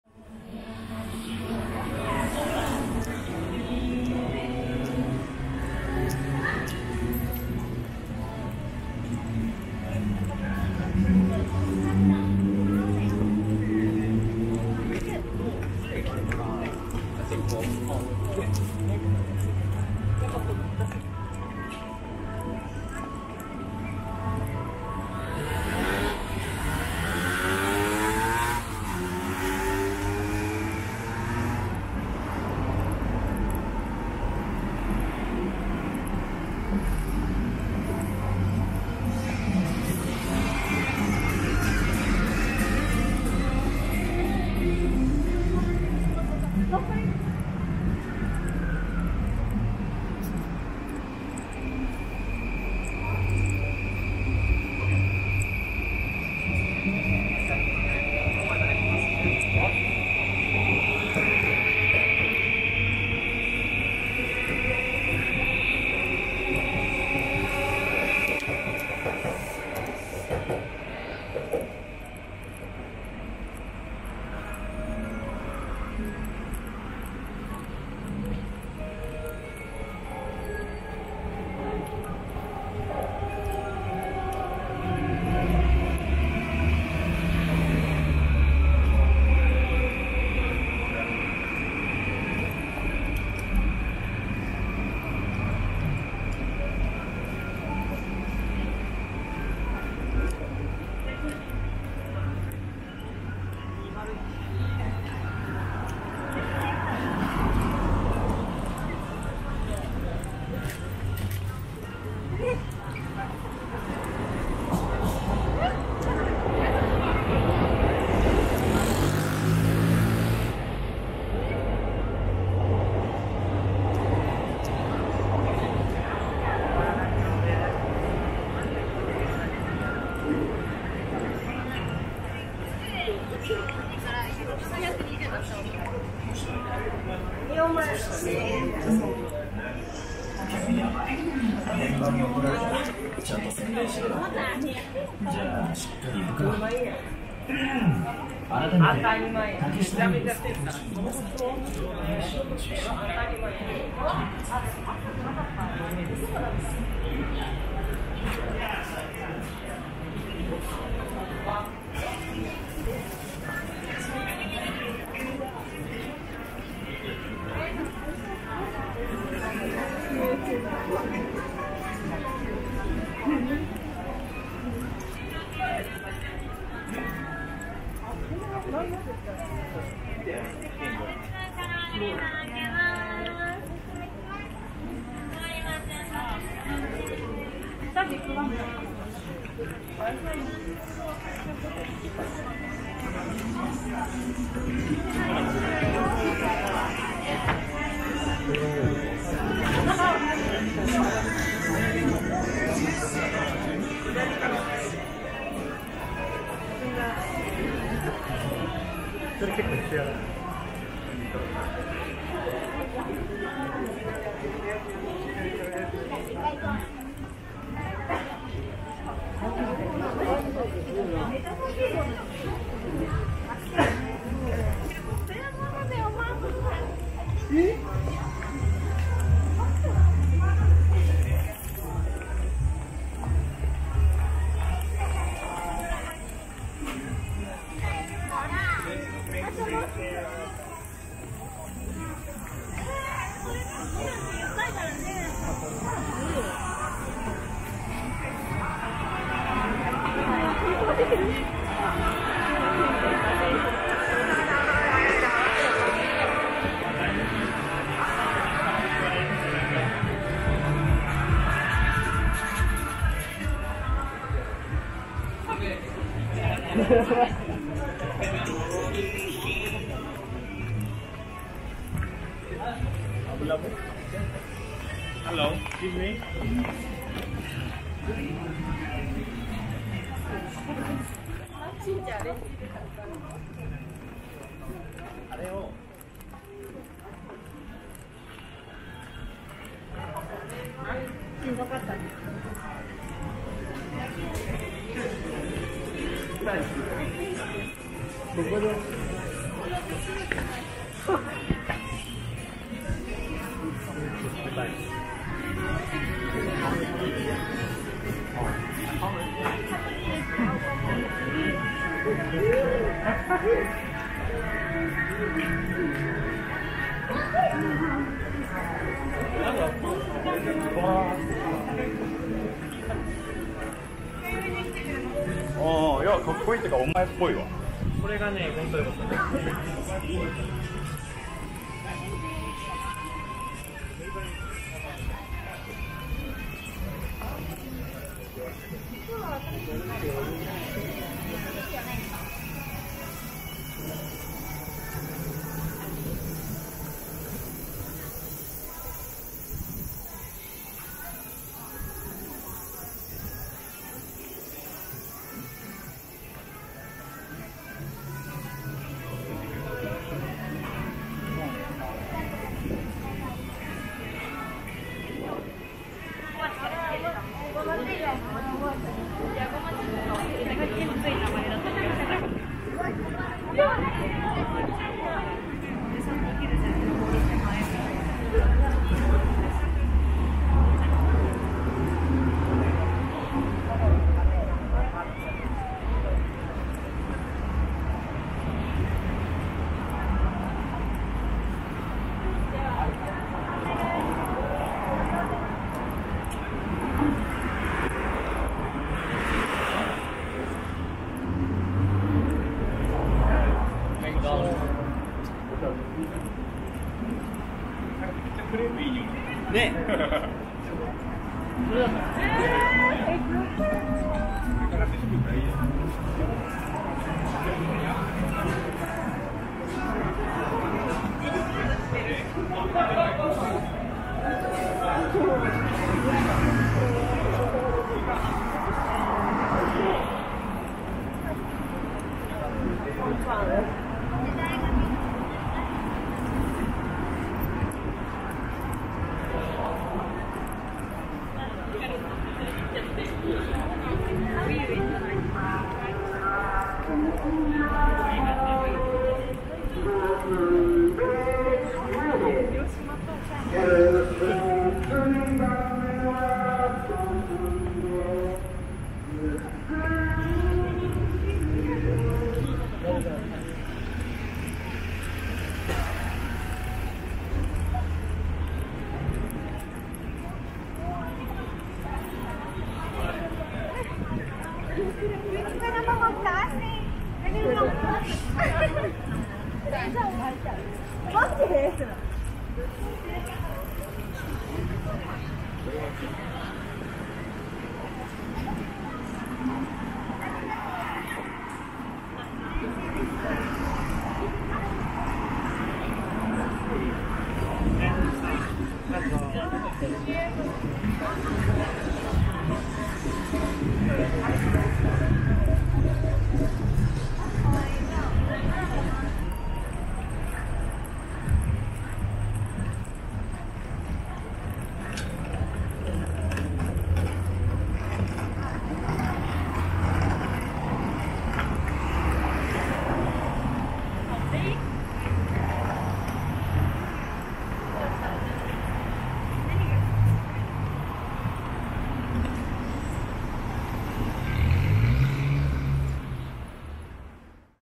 【4K】 Harajuku Sta. Takeshita St. 東京散歩 东京 도쿄 가이드 Walking Tokyo 2022 Jul.
I went to Harajuku Takeshita street.
Please enjoy my channel.